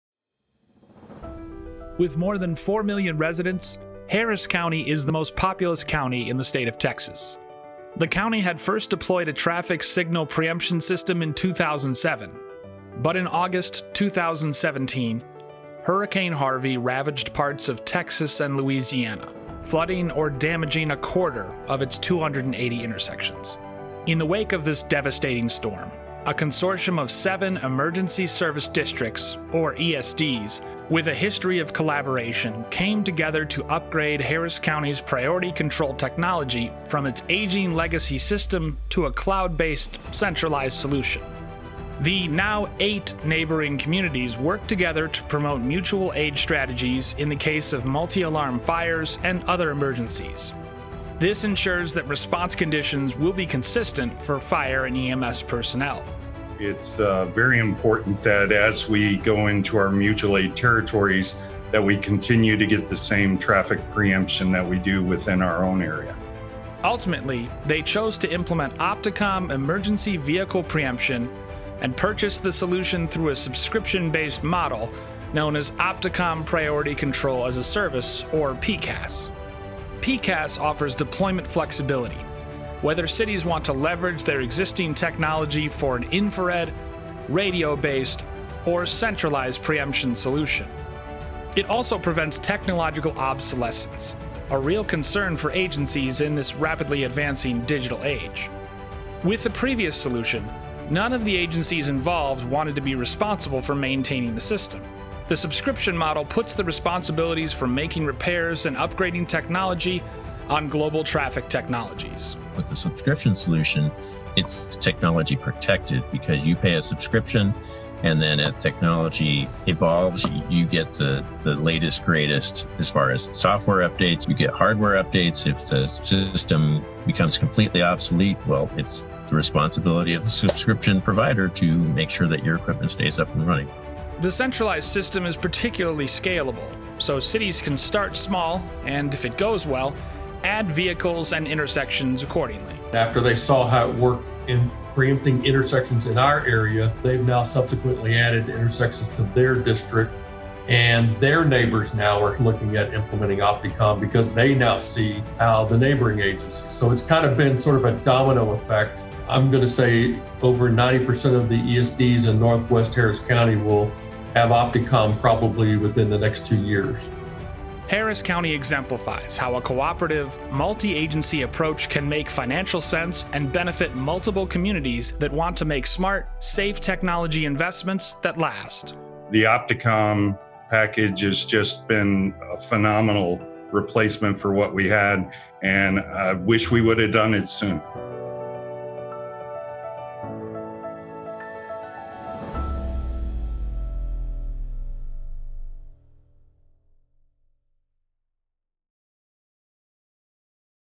Harris County Upgrades to Centralized Preemption from Opticom
With more than four million residents, Harris County is the most populous county in the state of Texas. The county had first deployed a traffic signal preemption system in 2007. But in August 2017, Hurricane Harvey ravaged parts of Texas and Louisiana, flooding or damaging a quarter of its 280 intersections. In the wake of this devastating storm, a consortium of seven Emergency Service Districts (or ESDs) with a history of collaboration came together to upgrade Harris County’s priority control technology from its aging legacy system to a cloud-based, centralized solution in Opticom Priority Control as a Service from GTT.